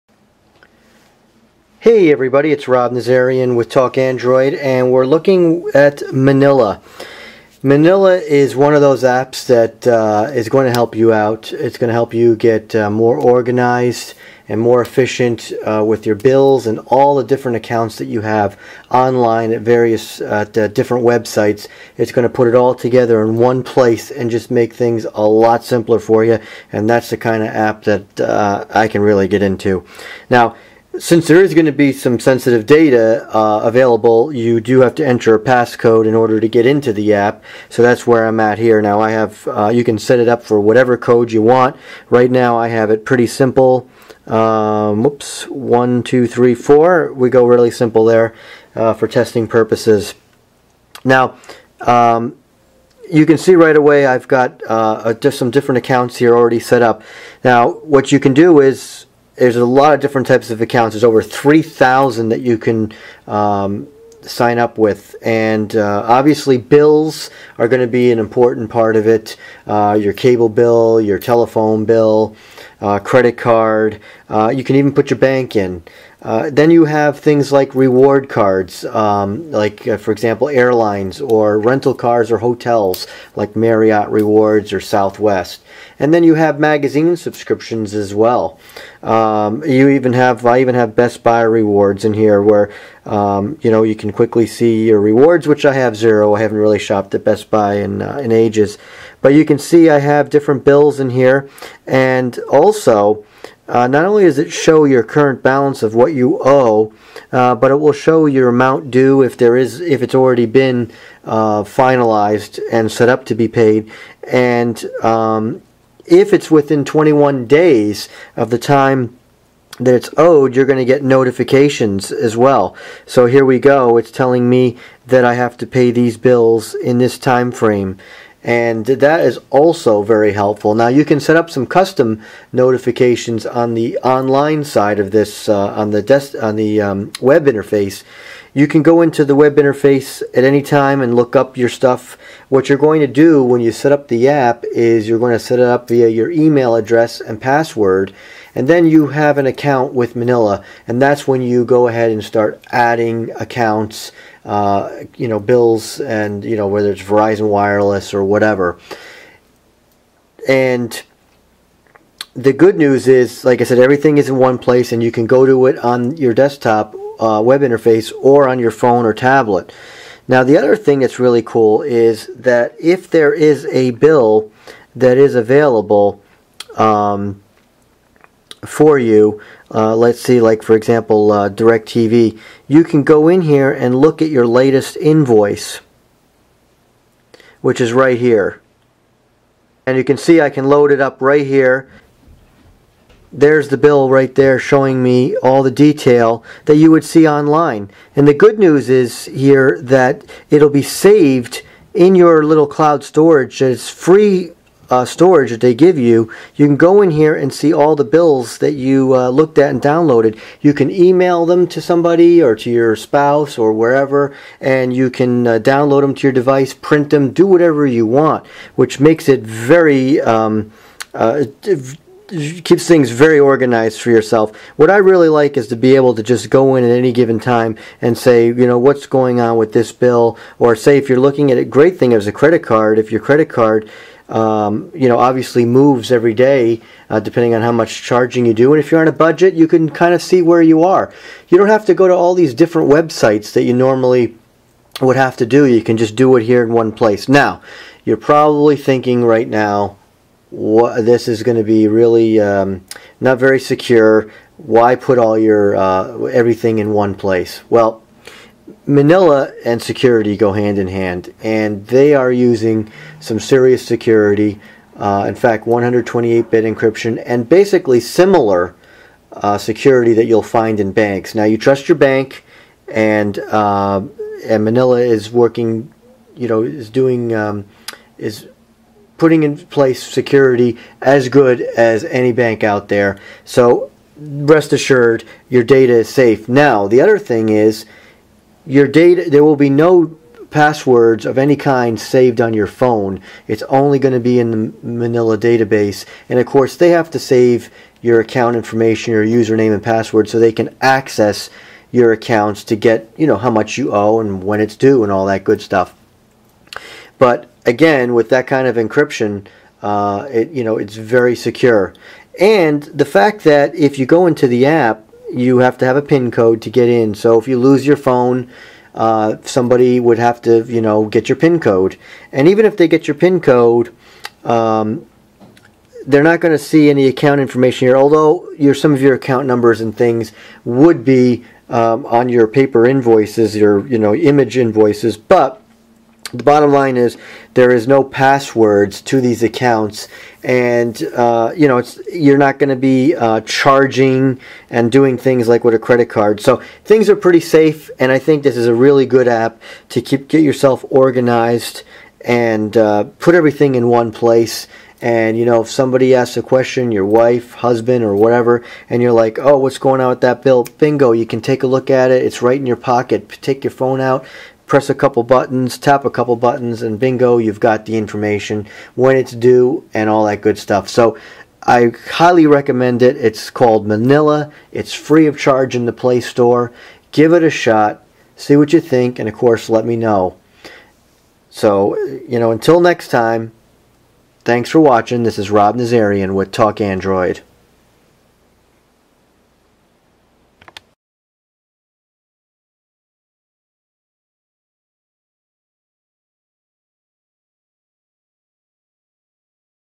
Hands on with Manilla Bills and Reminders for Android
Manilla helps you manage your bills, finances, travel rewards and subscriptions.